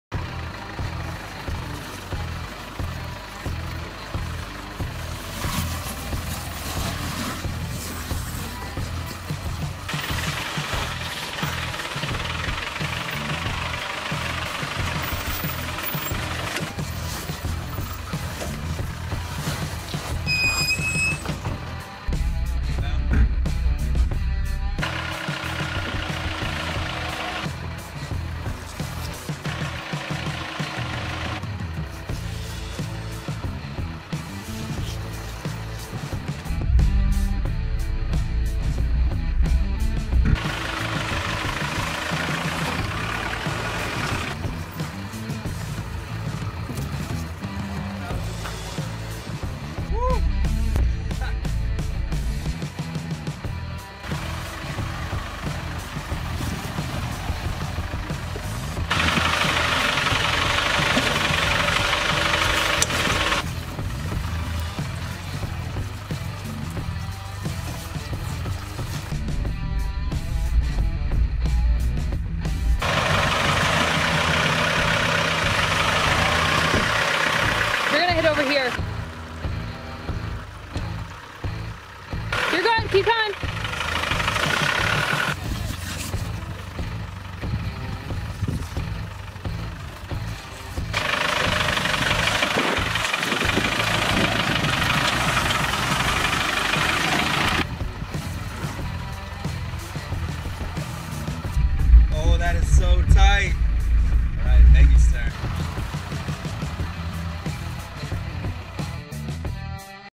Defender 90 on 37s on Log Corral Trail, Arizona
My Defender 90 on the first two obstacles on Log Corral Trail, Arizona.

Enjoy the short video!